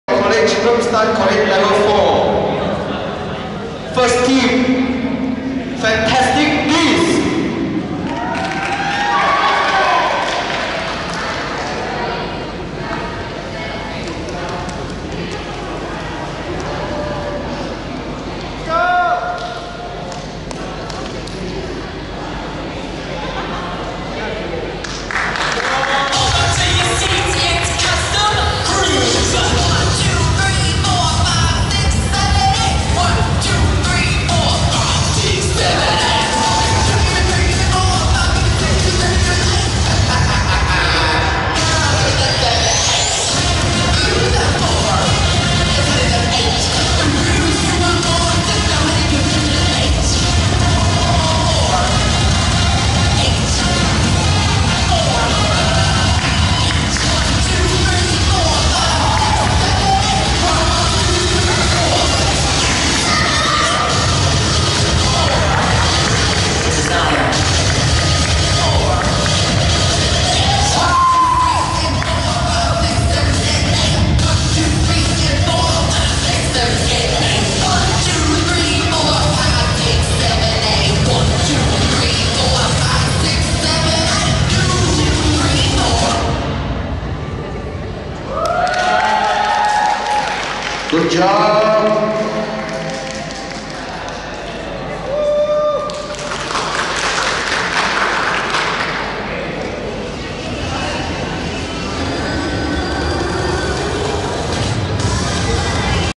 27 Fantastic beast, University or College Stunt Coed Level 4, Hero Showdown 2019 - Cheerleading
27 Fantastic beast, University or College Group Stunt Coed Level 4, Hero Showdown 2019 - Cheerleading

Hero Showdown 2019 - Cheerleading
at Dwi Emas International School
No. 5, Jalan Ikhtisas 14/1,
Seksyen 14, Pusat Bandar Shah Alam,
40000 Shah Alam, Selangor